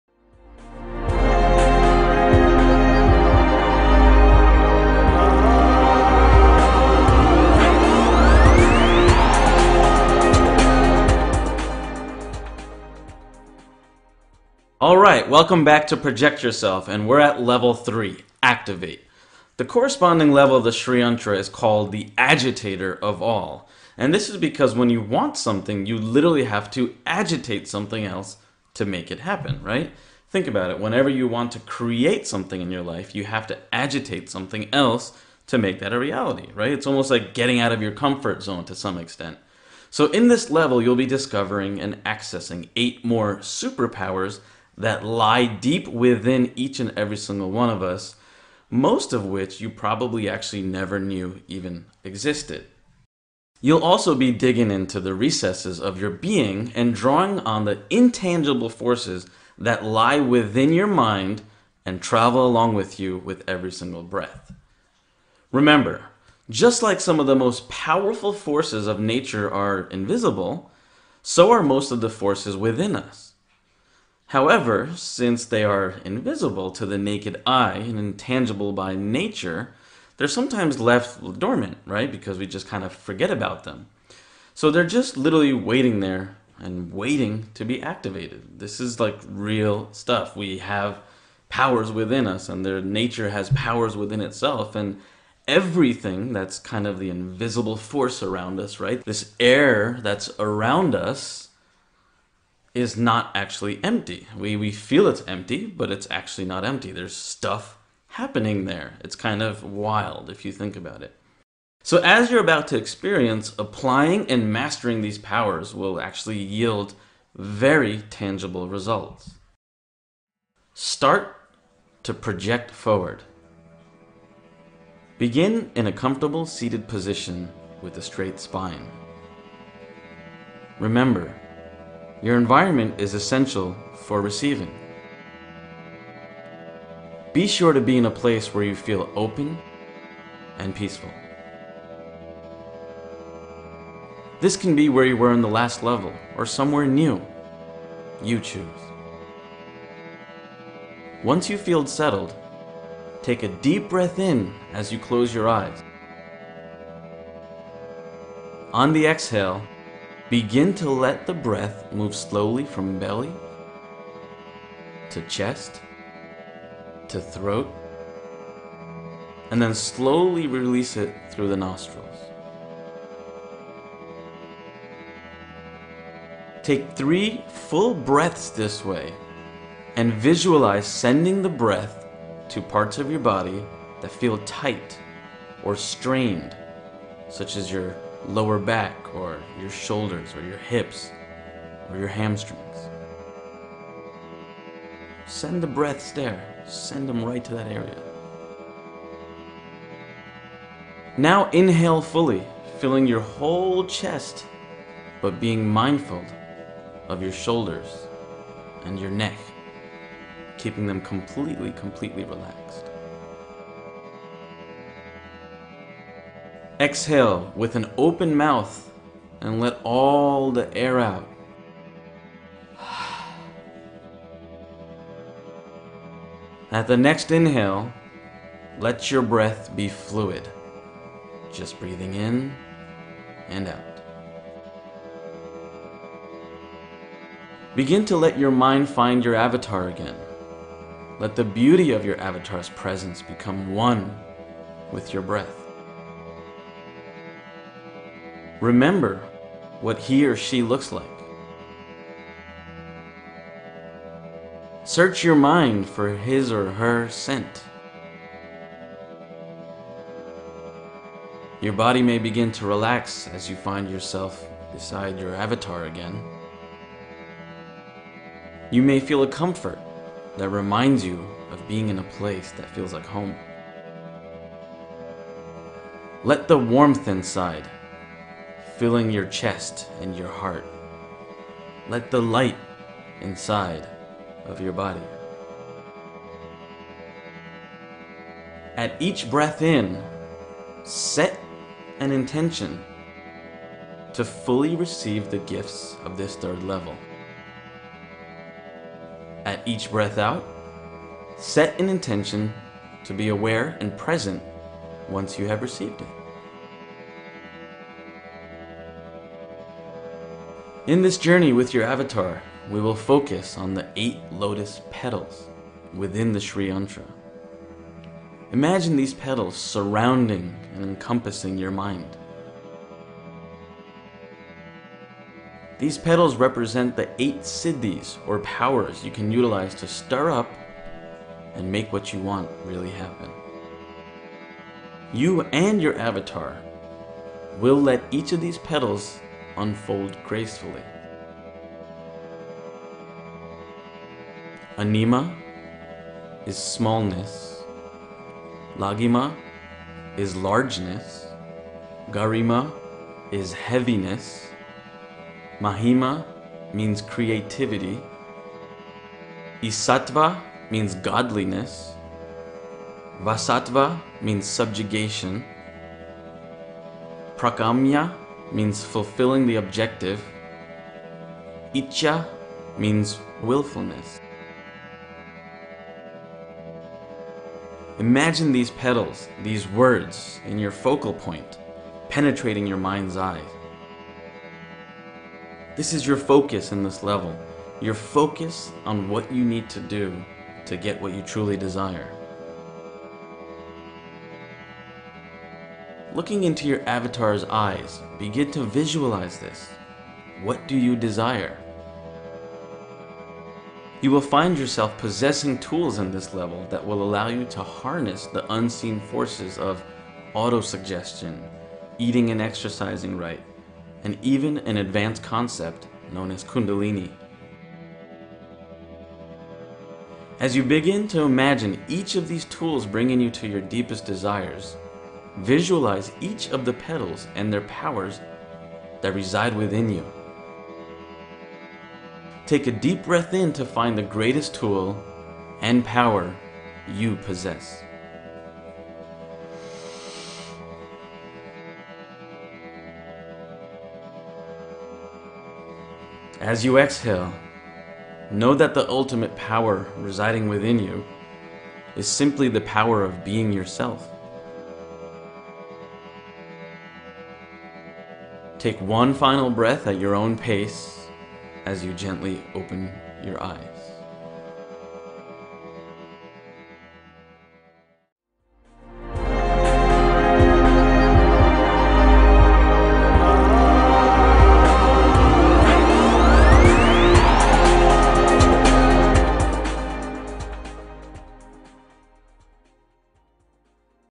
Guided Meditation to Getting Out of Your Comfort Zone
A guided meditation to get out of your comfort zone, because this is how to change your life, this is how you create a new “you”, a better version of yourself. You’ll discover of how to access more superpowers. Just as some of the most powerful forces of nature are invisible, so are the most powerful forces within you. However, since they are invisible to the naked eye and intangible by nature, they are sometimes left dormant...just waiting to be activated.

Please check out the link to learn more about how to change your life for the better and unleash your maximum potential!